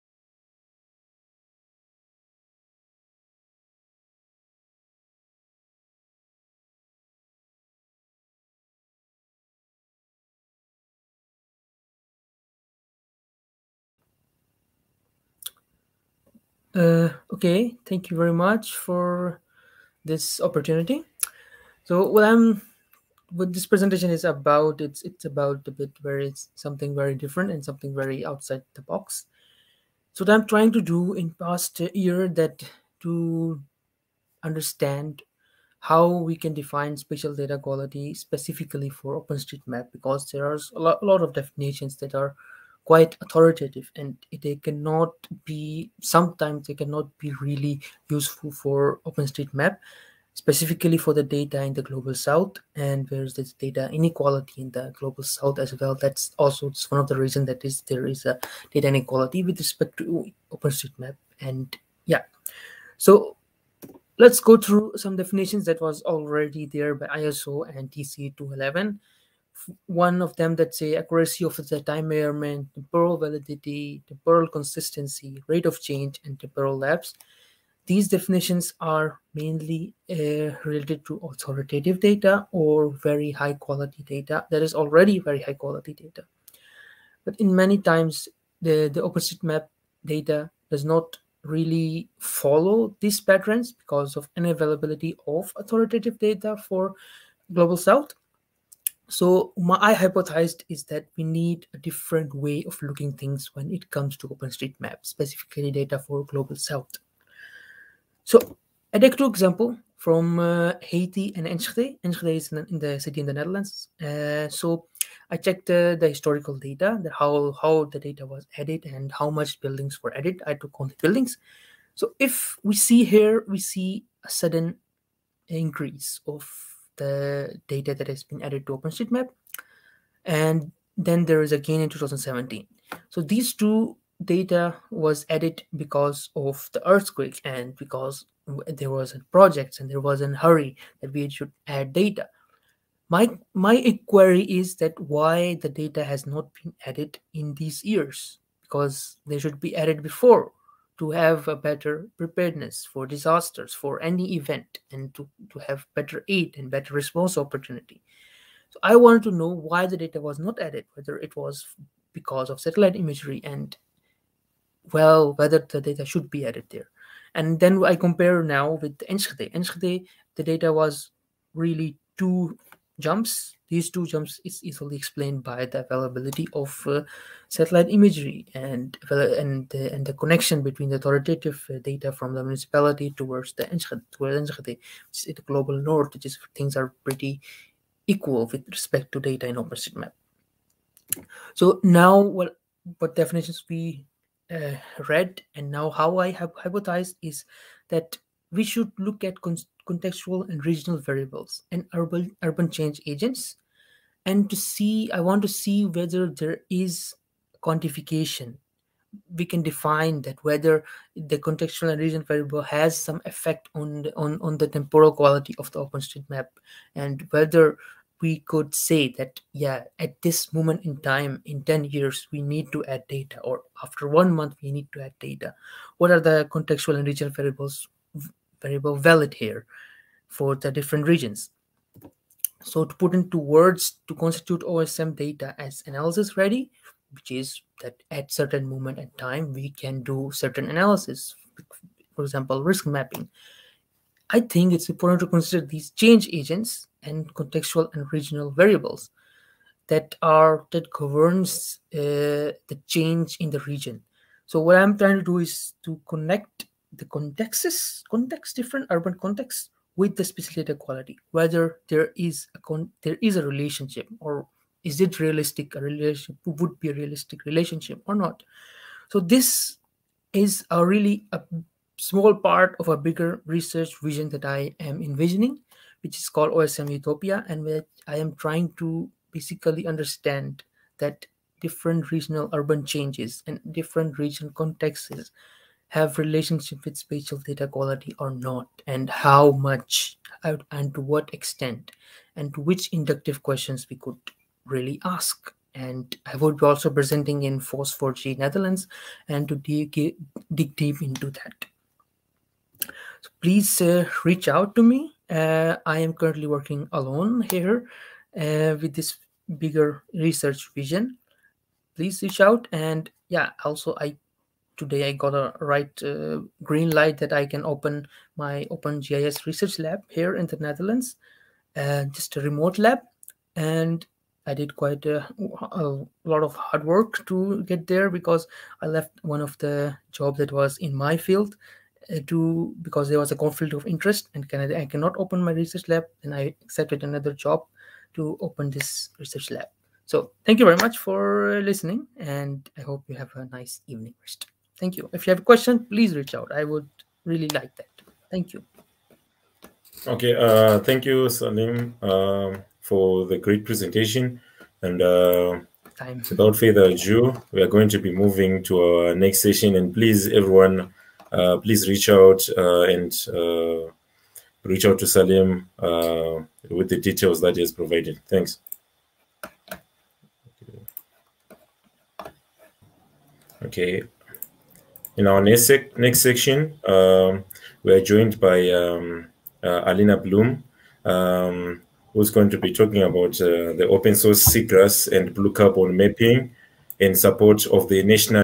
FOSS4G 2021 - Defining temporal spatial data quality aspects for OpenStreetMap
Defining temporal quality for OpenStreetMap comes with additional challenges while assessing the data from global south. This is because the extrinsic quality measure cannot be taken, and intrinsic measures can only indicate subjectively where the data is doubtful. This work take the challenge to define the temporal quality aspect for the purpose of disaster risk reduction that would determine when data should be updated, revisited, and produced and when it can be constituted as incomplete in OSM. This presentation is part of larger research aim that I as an independent researcher am trying to achieve which developing a relationship between (regional) contexts and OSM data quality.

The OSM data is staled and in many instances not updated. The example of OSM data production in Haiti is an interesting and alarming case because the data is only produced when there are distresses. While if we visualize the pattern for data production from OSM history viewer from Heidelburg institute we can see that there are sudden jumps of data production which has correlation with major HOTOSM project and/or major disaster. This causes alot of back log in aid and situational awareness. The data needs to be added before urgency. This work will try to work on finding solutions on how can temporal uncertainty be detected.

Muhammad Saleem,
Open GIScience Research Lab, Enschede, the Netherlands

Track –
Transition to FOSS4G

Topic –
FOSS4G implementations in strategic application domains: land management, crisis/disaster response, smart cities, population mapping, climate change, ocean and marine monitoring, etc.

Level –
1 - Principiants. No required specific knowledge is needed.

Language of the Presentation –
English